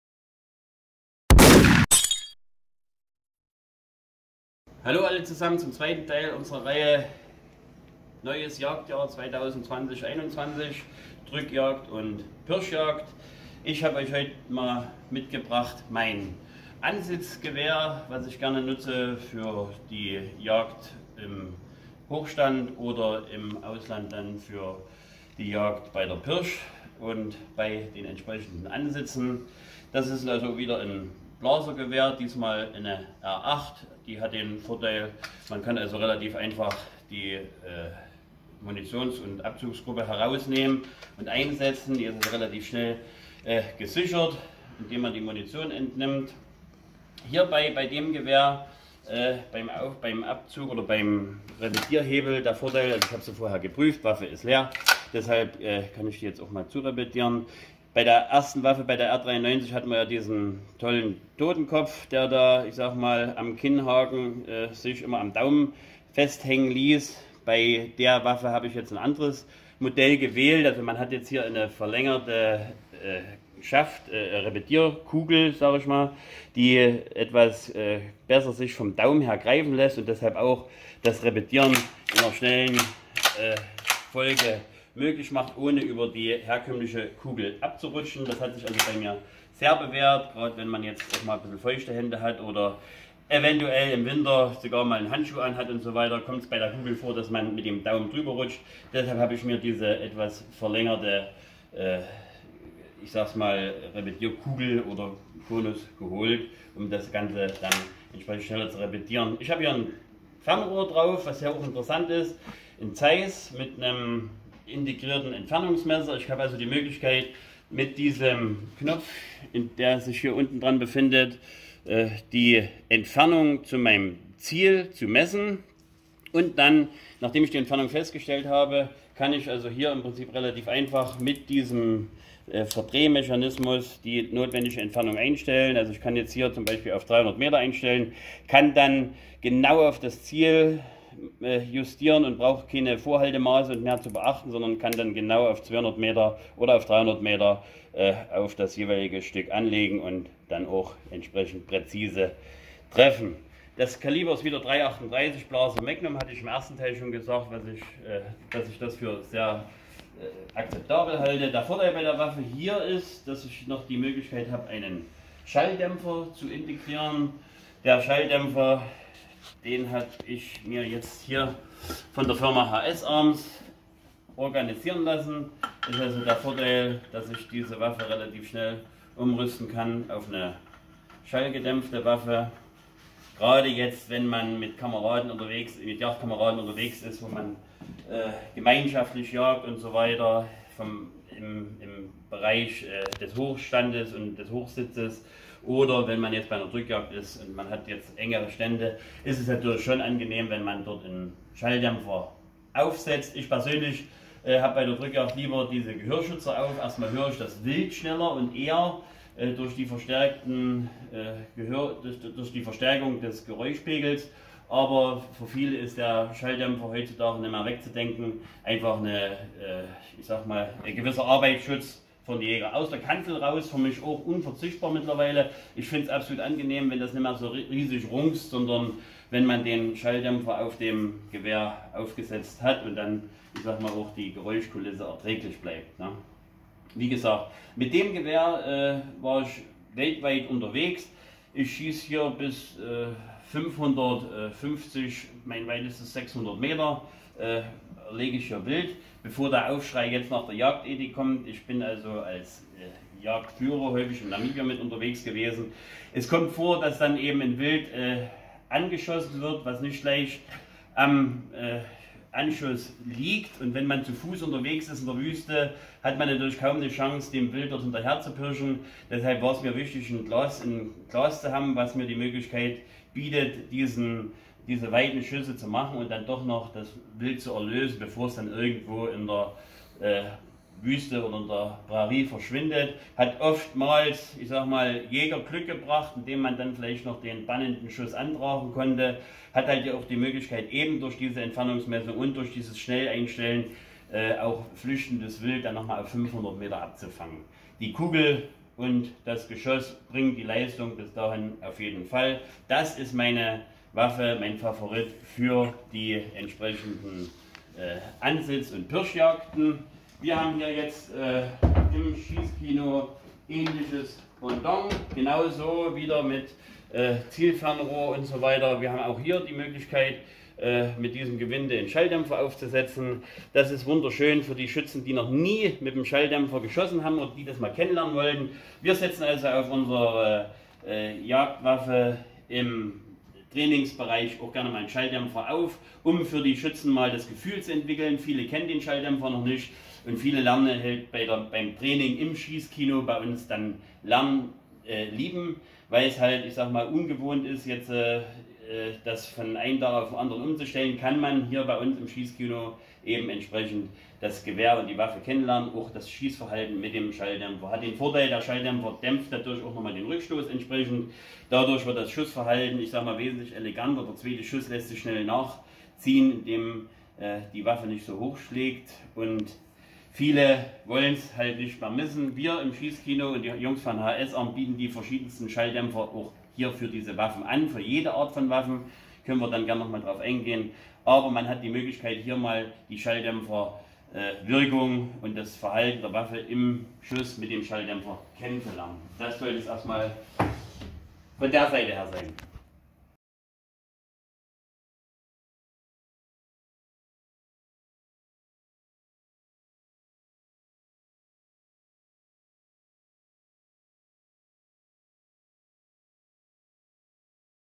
Jagdtipps vom Jäger für Jäger - Teil 2 - Thema: Gewehre für Ansitzjagd
Beginnend zur Jagdsaison praktische Tipps zur Jagd von einem erfahrenen Jäger.